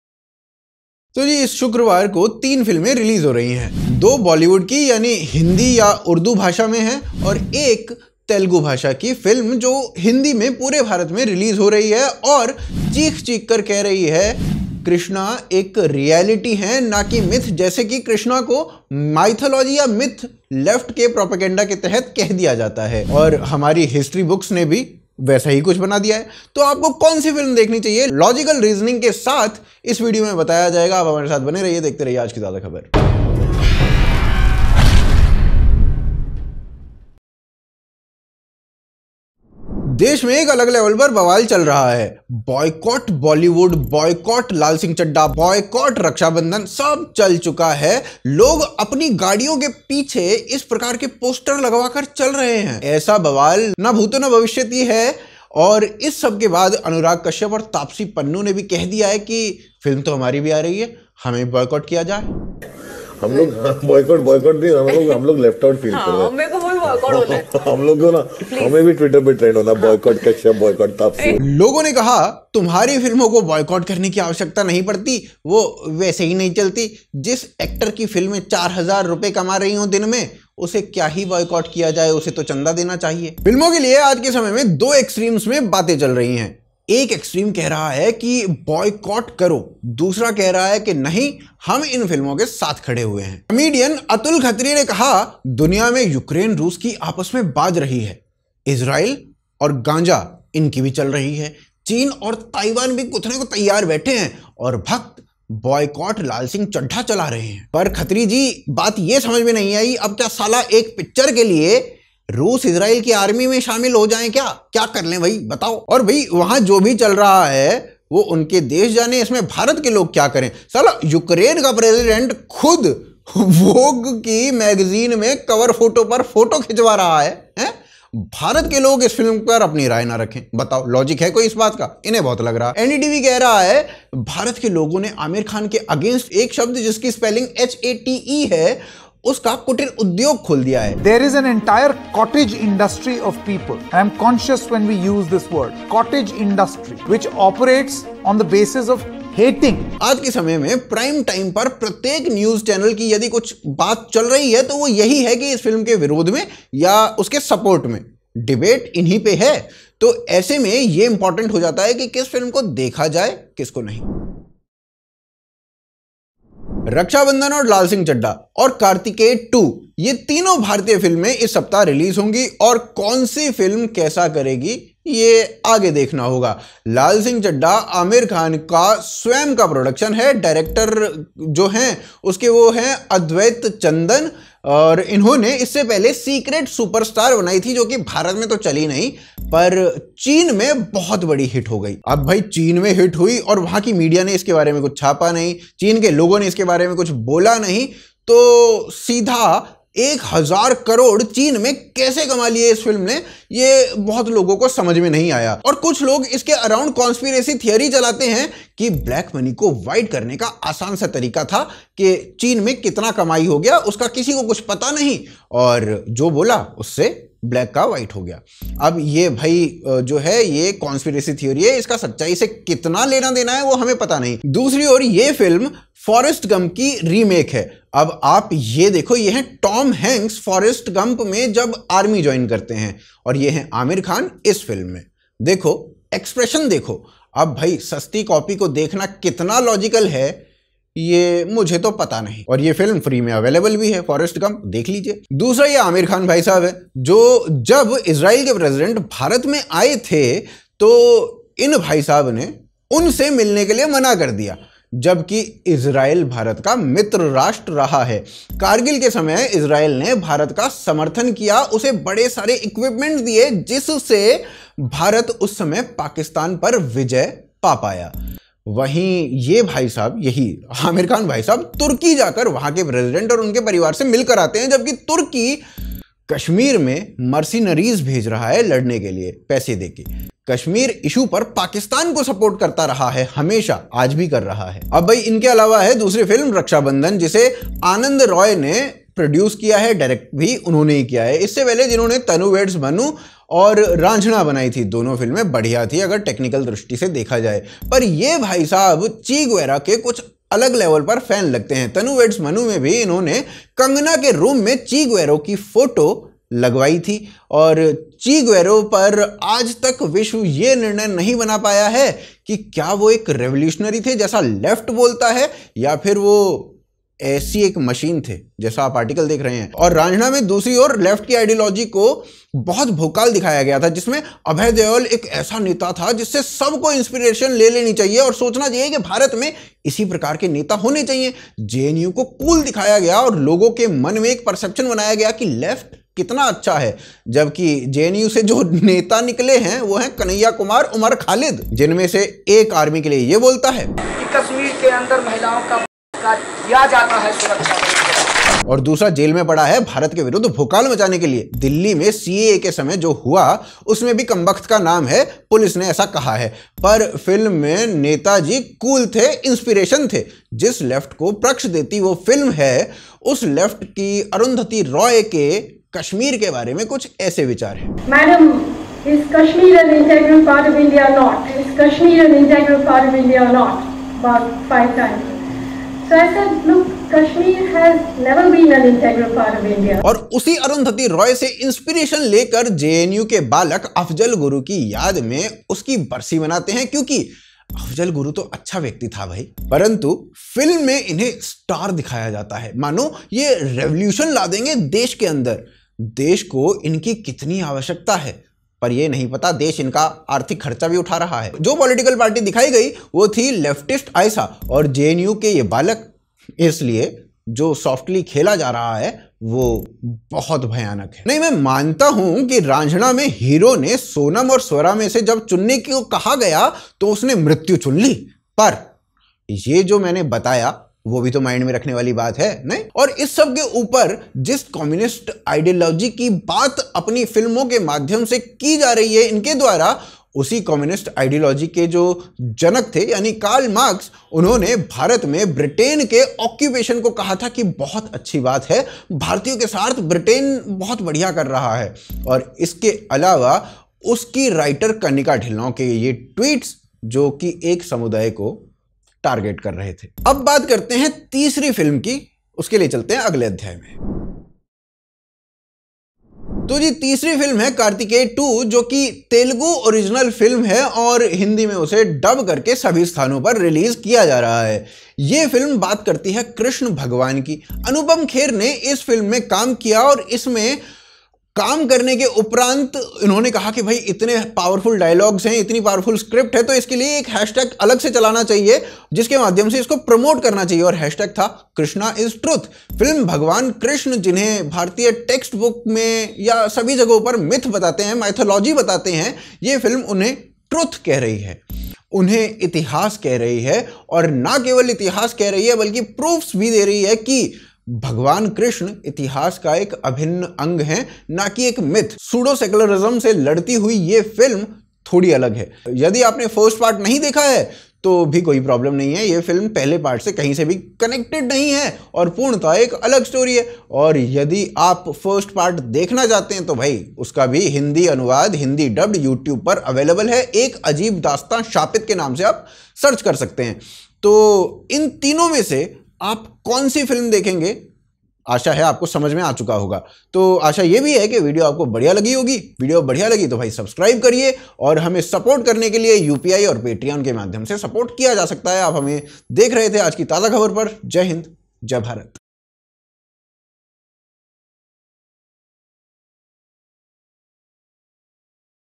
This Movie has shaken Lal Singh Chaddha's Plan | AKTK
Aaj ki Taza Khabar Youtube Channel, In this Video-
we will discuss the 3 films that are releasing on this friday, namely, Rakshabandhan, Lal Singh Chaddha & Karthikeya 2.